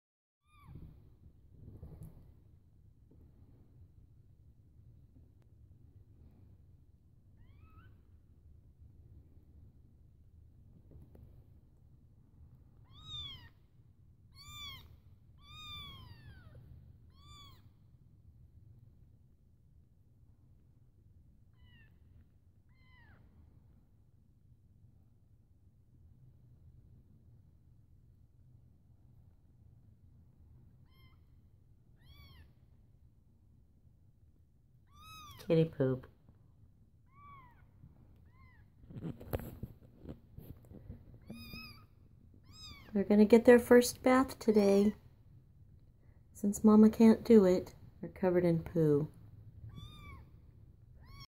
Basket of Adorable Orphan Kittens
Neighborhood kids brought me these four kittens who didn't even have teeth yet, but without the mama. I am caring for them, while trying to trap the mama. They will get their first bath today, since they are covered in poo, and mama cat isn't here to clean them.